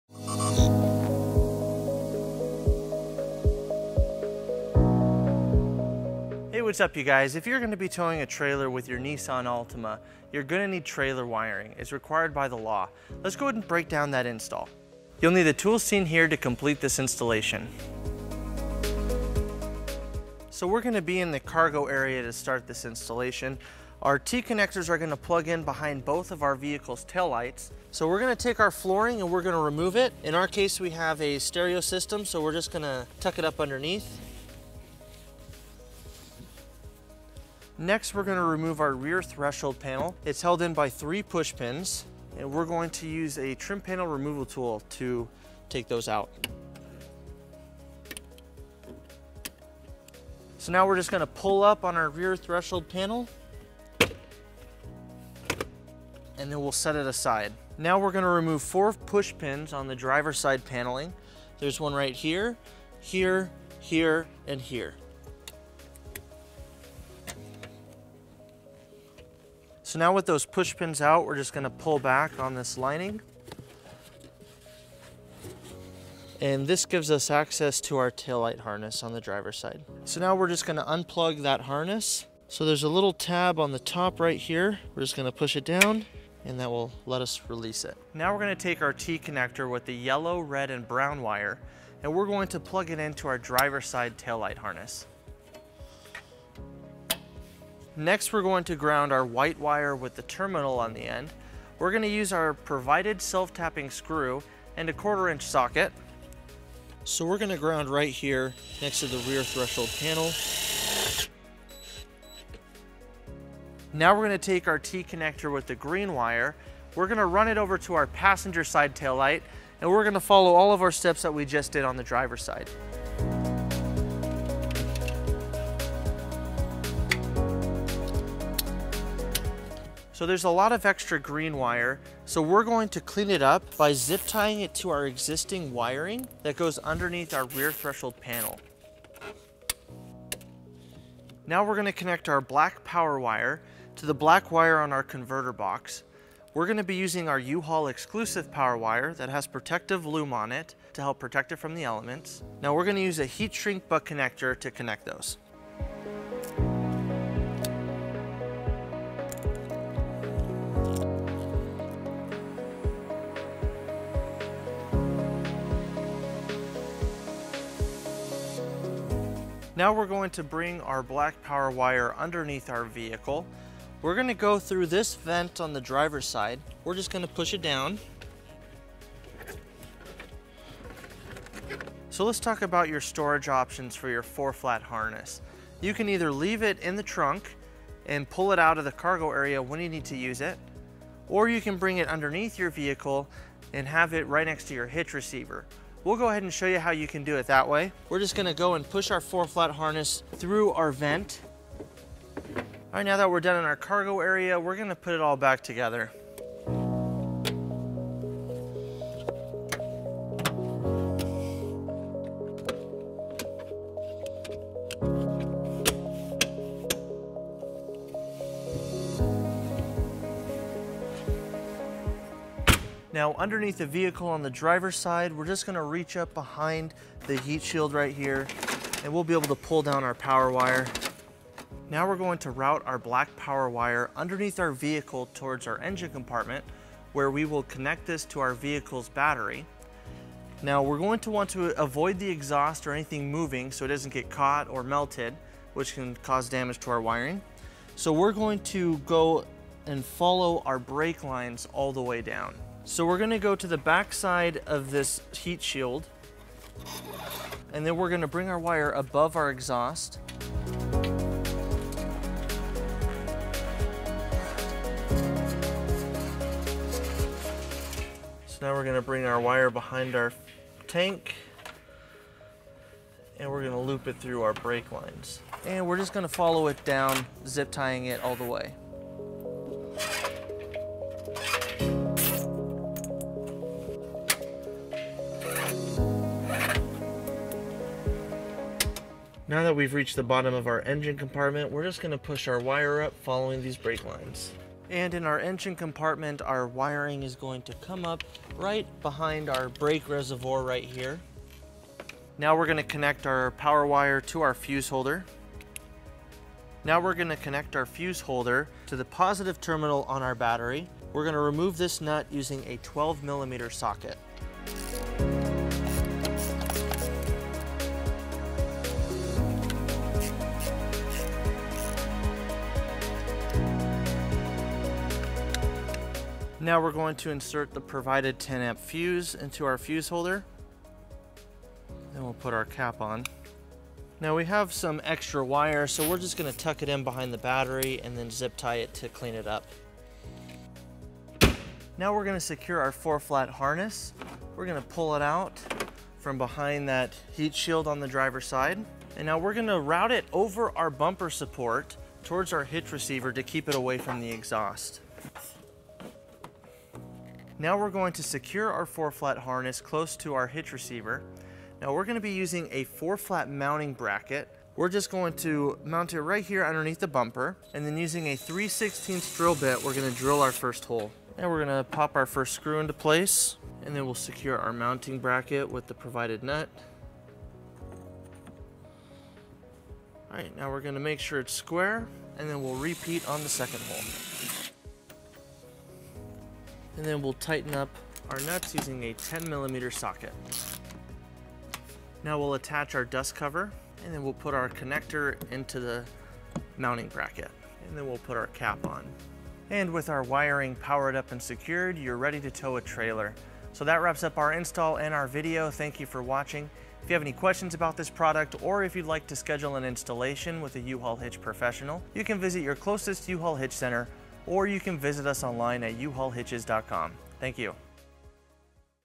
2018 Nissan Altima Trailer Wiring Harness Installation
This video covers the wiring harness installation for Curt 56339 on a 2018 Nissan Altima. Learn how to install the trailer wiring on your 2018 Nissan Altima from a U-Haul installation professional. With a few tools and our step-by-step instructions you can complete the installation of Curt 56339.

To purchase this wiring harness and other towing accessories for your 2018 Nissan Altima,

0:00 Intro
0:15 Tool list
0:20 Remove cargo panels
1:20 Install wiring harness
3:23 Reinstall cargo panels
3:43 Route power wire to battery
5:41 Install connector bracket
6:40 Outro